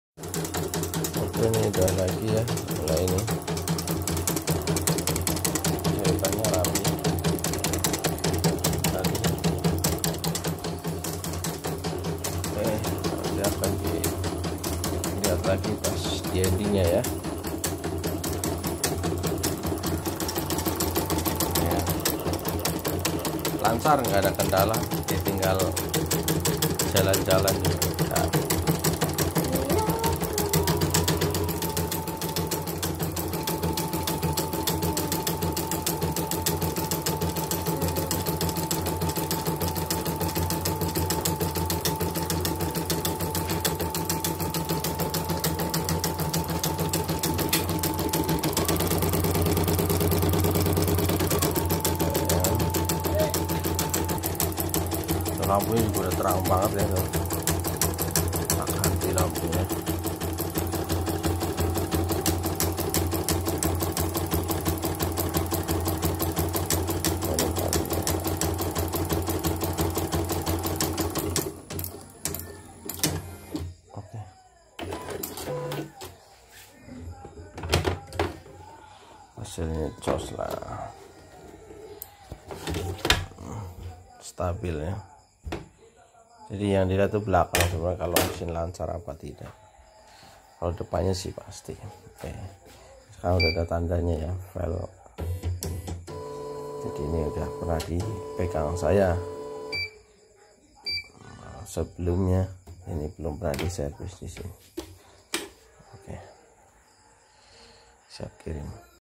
M8 Mesin bordir macet Mati total - KIAN PERKASA SCMB
Tahapan perbaikan mesin M8 Mesin mati total